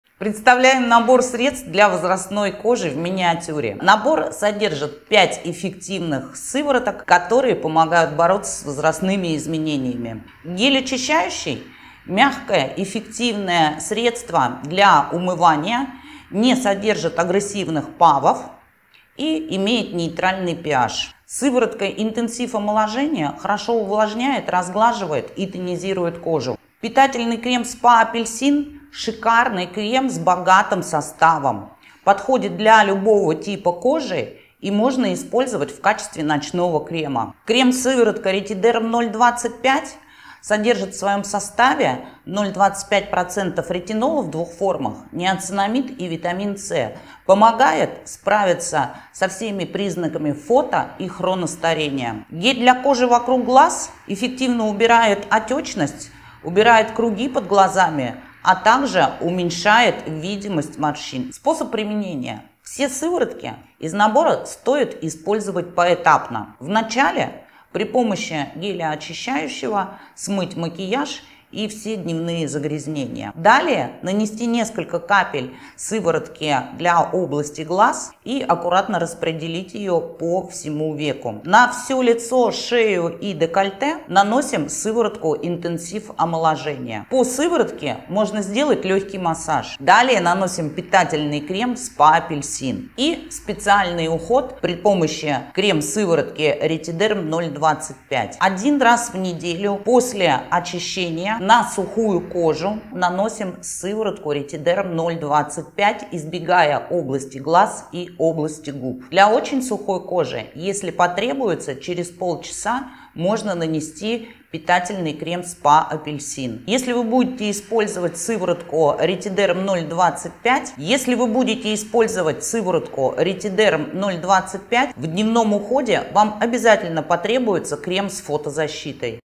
Набор средств для возрастной кожи в миниатюре
Набор включает в себя миниатюры пяти средств, каждое из которых помогает продлить молодость кожи и обеспечить сбалансированный уход. Каждое из них работает в конкретном направлении, решая проблему и нейтрализуя разные причины ее появления - такие, как обезвоженность, сухость, отечность, потеря упругости кожи, признаки фото - и хроностарения.
Результатом от применения гелей, сывороток и крема из этого комплекта станет более обновленная и увлажненная кожа, сокращение морщин, а также снижение отечности и повышение упругости кожи.
Кроме того, покупка набора из миниатюр станет прекрасной возможность подобрать средство, наиболее подходящее лично вам, ведь каждый из нас - индивидуальность и имеет свои требования.

НОВИНКИ! ______________________________________